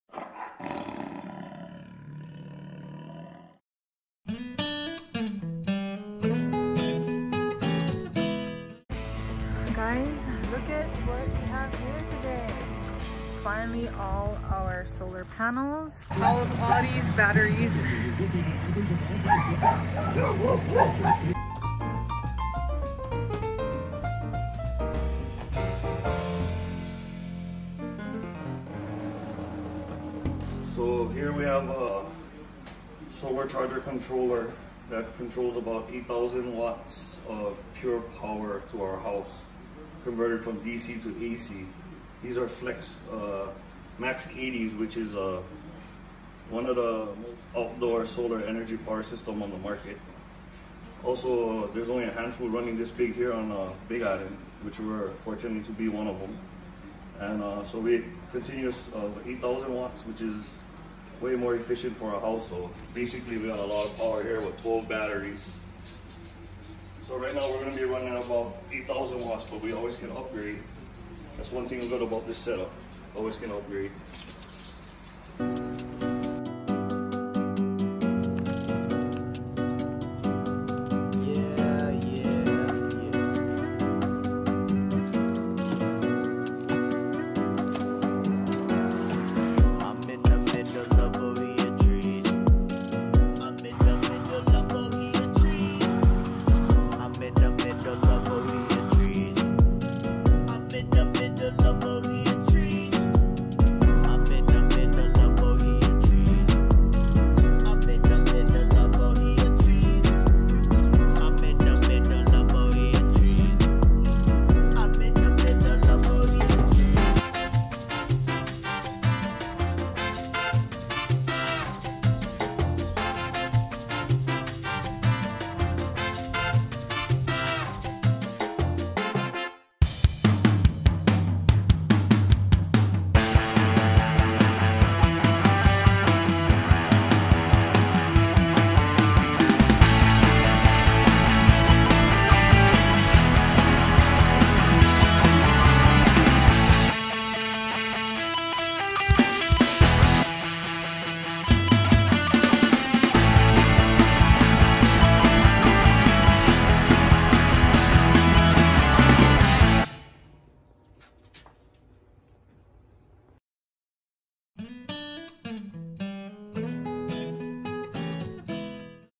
Solar Set Up | OFF GRID LIFE | Homestead Update
Finally have our solar set up installed for our offgrid home.
Purchased from Ultimate Off Grid Solar on the Big Island of Hawaii.

Shout-Out to the one and only:
@Sin G

16-566 Keaau Pahoa Road
Box 188-434
Keaau, HI 96749